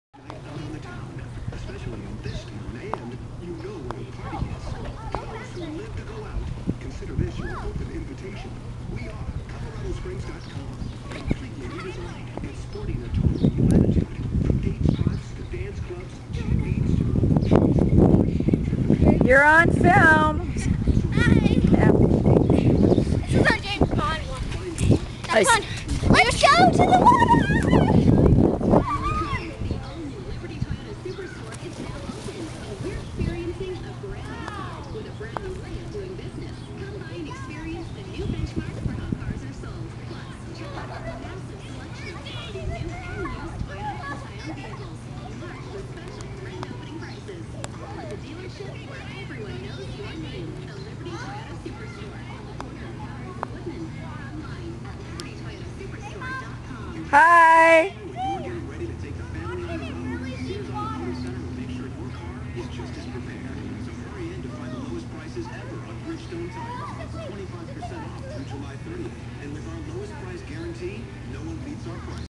Beach 7-21-09
Jo Ann took Annabelle and her friend Hannah Johnson to the beach at Prospect Lake in Colorado Springs on 7-21-09.  We had a great time and need to go there more!